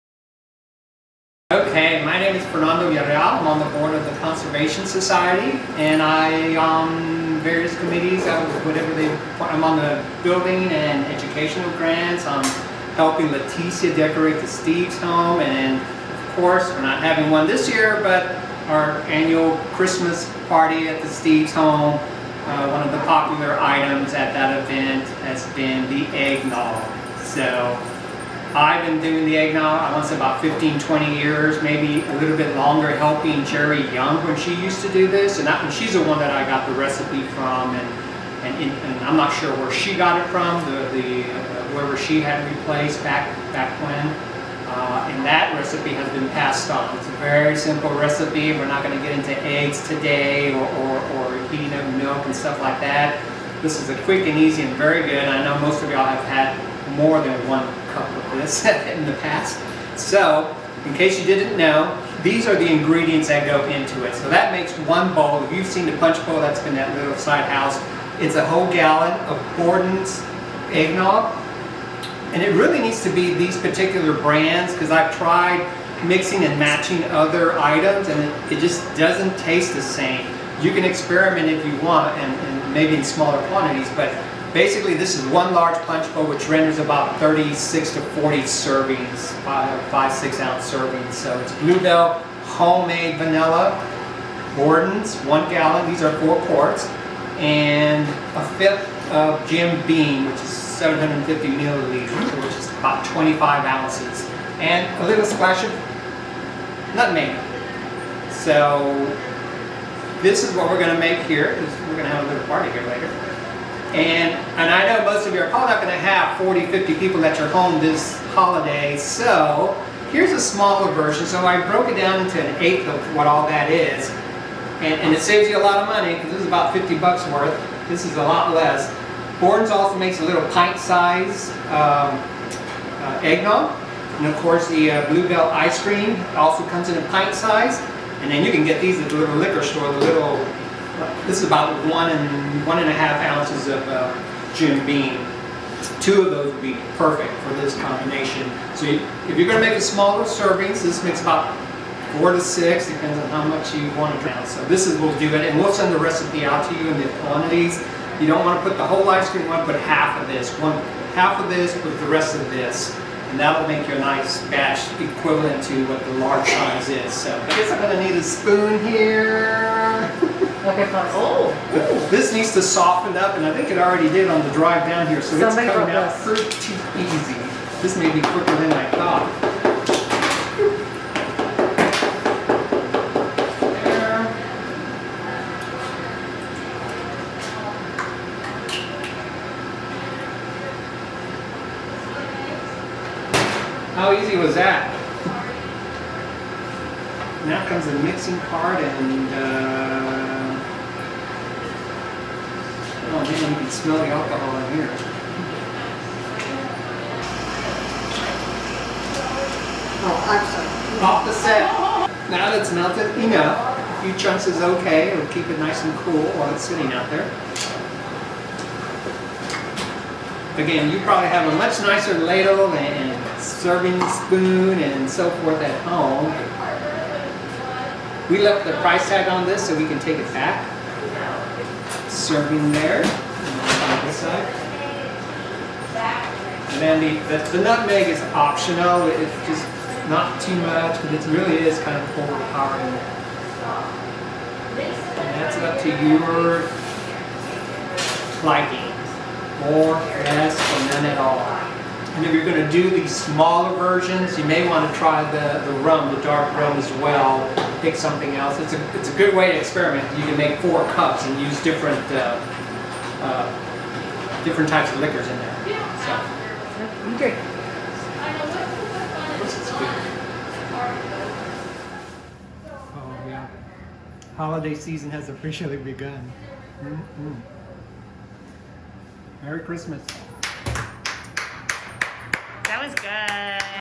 Weihnachtsfeier December 9th – Fernando Villarreal
Society Weihnachtsfeier Christmas Party - December 9th – Fernando Villarreal provides a demonstration of making a traditional holiday favorite beverage: Eggnog